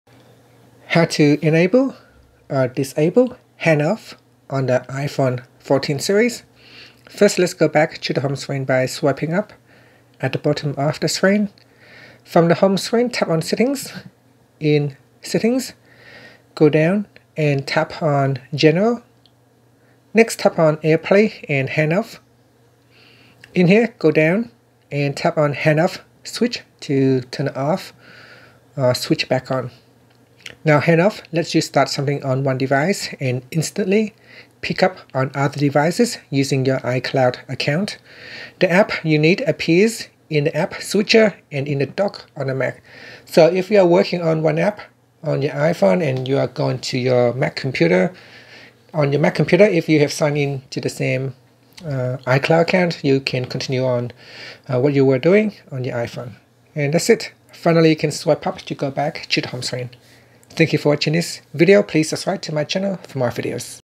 iPhone 14's/14 Pro Max: How to Enable/Disable Handoff
Learn how you can enable or disable Handoff on the iPhone 14 / iPhone 14 Pro/14 Pro Max/ iPhone 14 Plus.